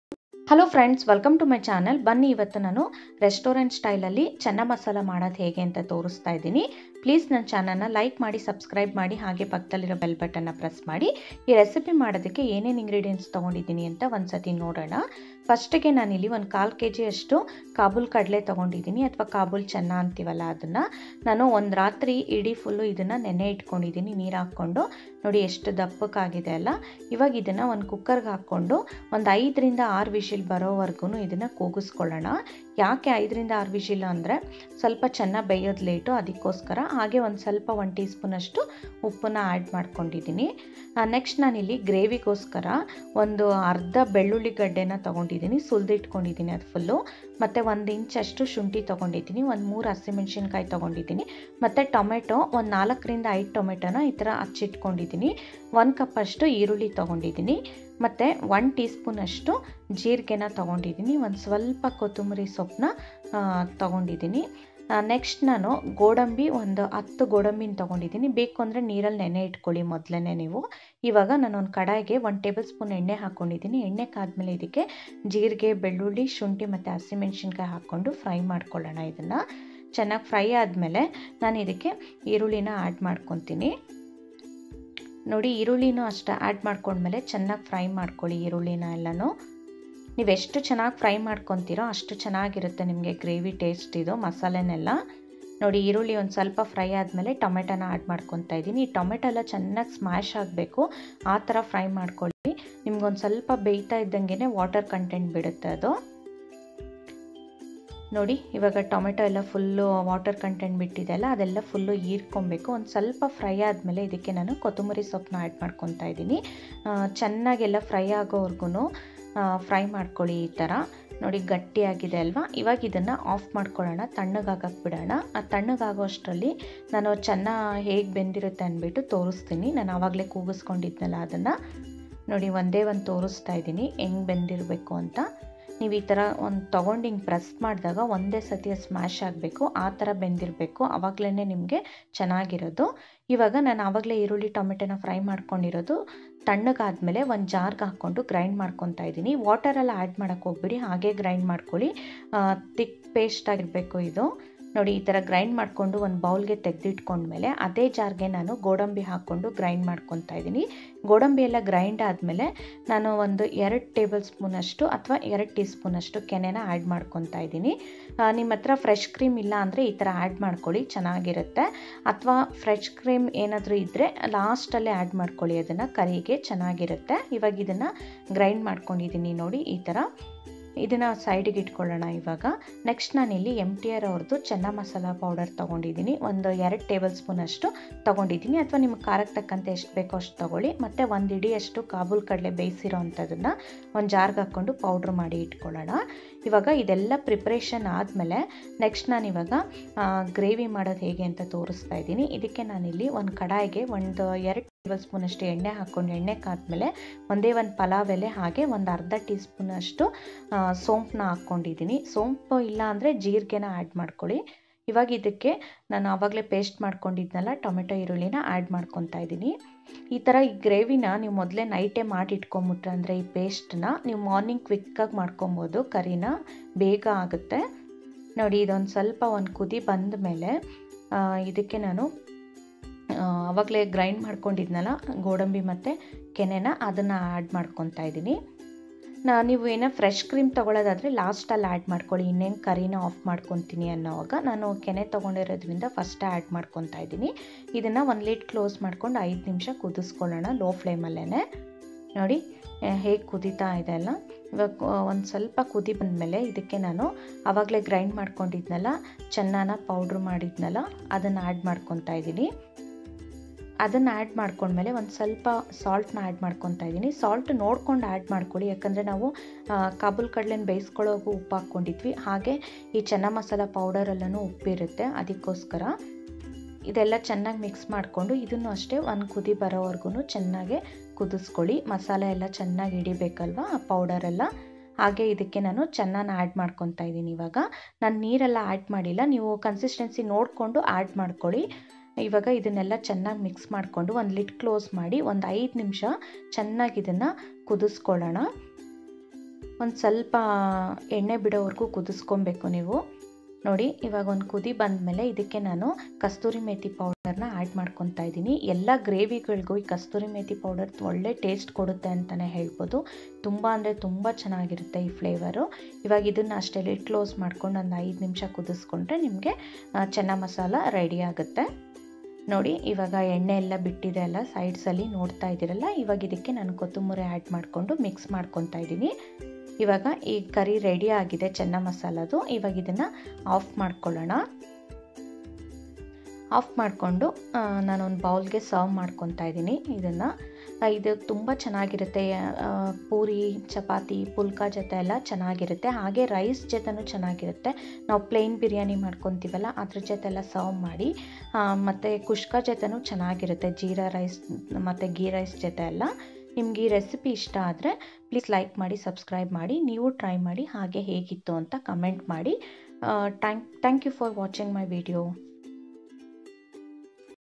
Restaurant Style Channa Masala | Channa Masla Gravy | Easy Channa Masala Recipe
Restaurant style Channa Masala Recipe can be made easly at home...
Best side dish for Chapathi, Pulka, Roti and with rice recipes...